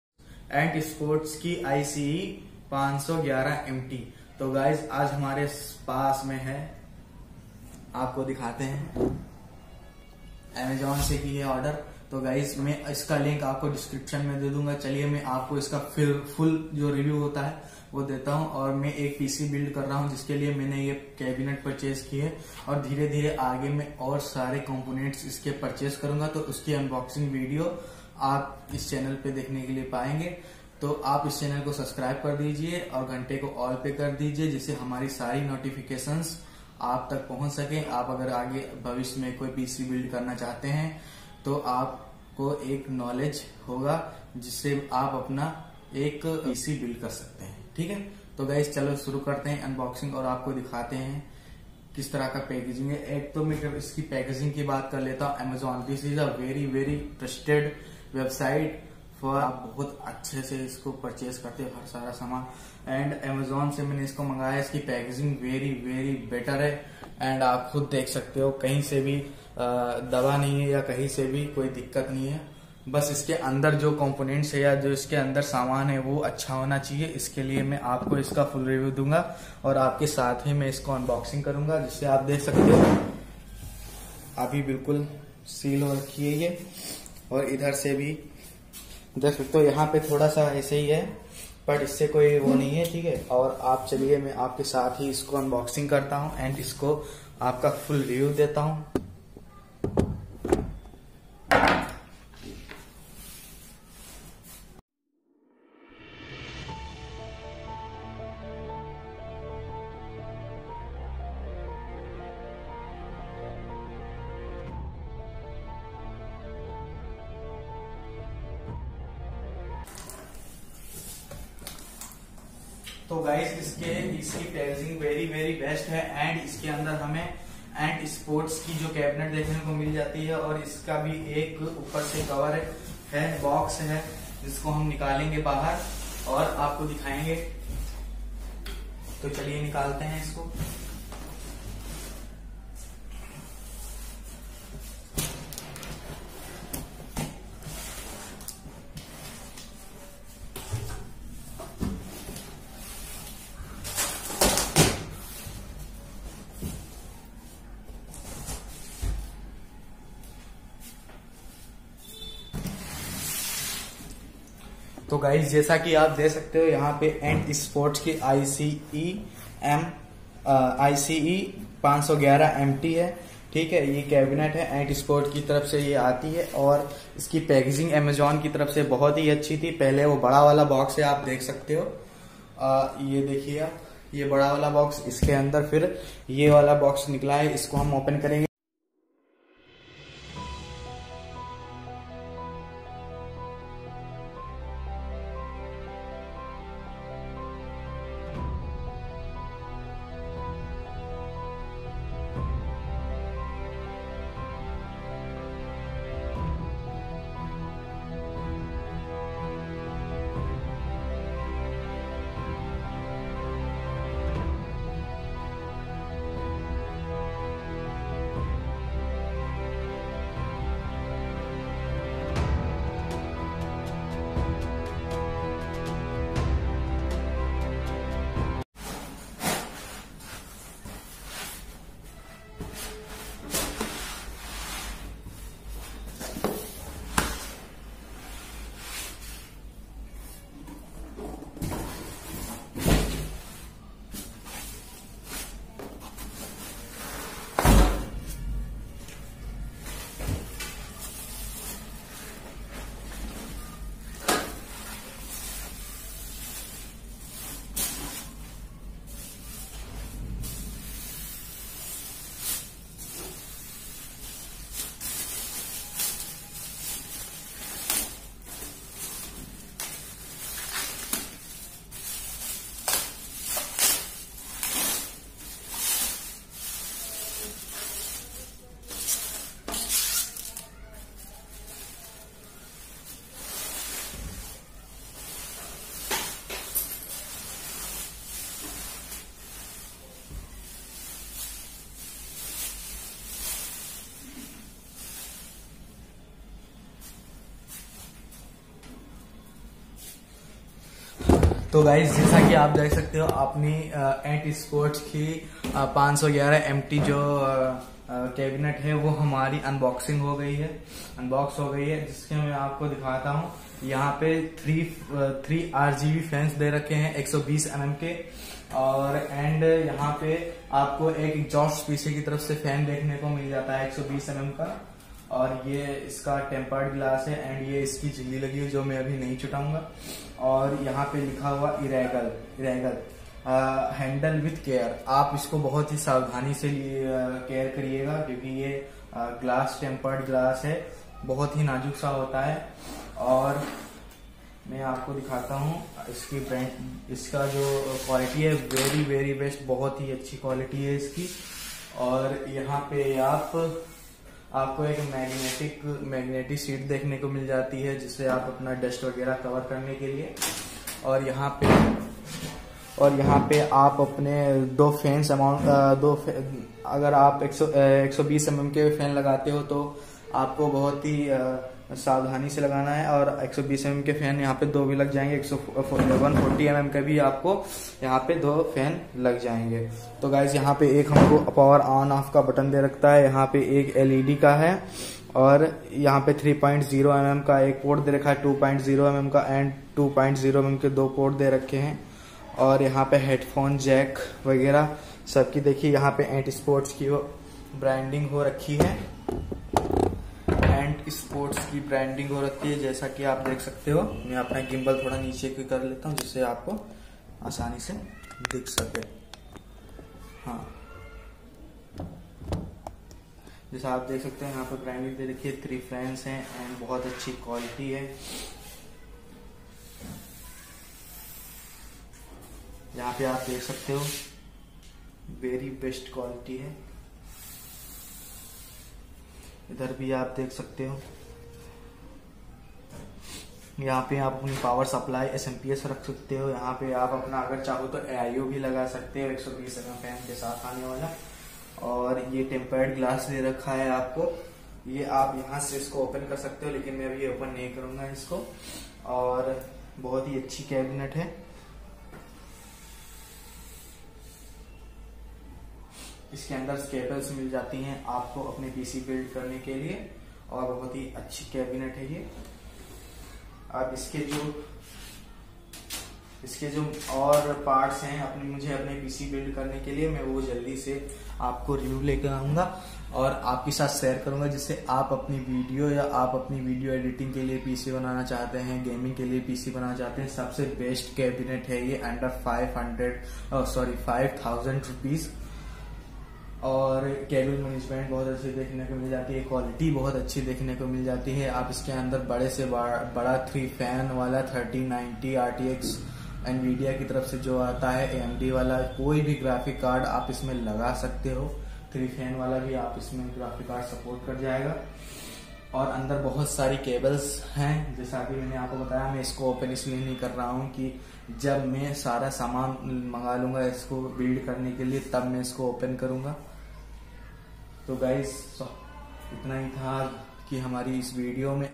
perfect cabinet for budget Gamers: ANT Esport 511MT
Best buy link :- Ant Esports ICE-511MT Mid Tower Mesh Gaming Cabinet Computer Case Supports E-ATX, ATX, Micro-ATX, Mini-ITX Motherboard with Sliding Tempered Glass Side Panel, 4 x 120mm Auto-RGB Front & Rear Fans